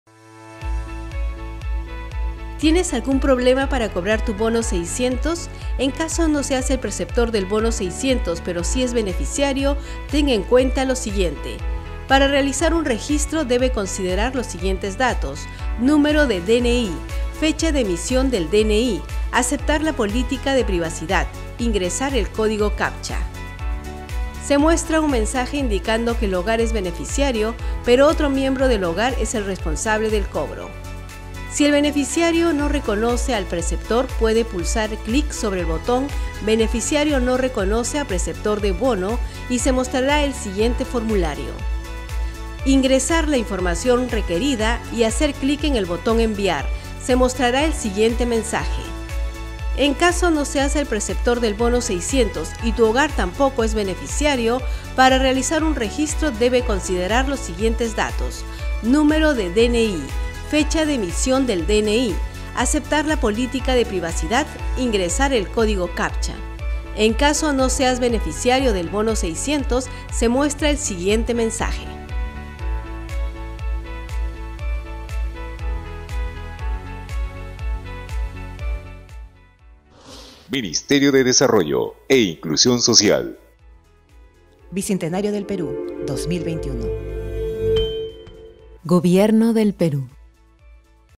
Bono600 | Hogar beneficiario
🏠 Si tu hogar es beneficiario del Bono600 y no eres el perceptor, y tienes inconveniente para cobrarlo, registra tu caso
✅ Recuerda que los bancos no dan información sobre el bono.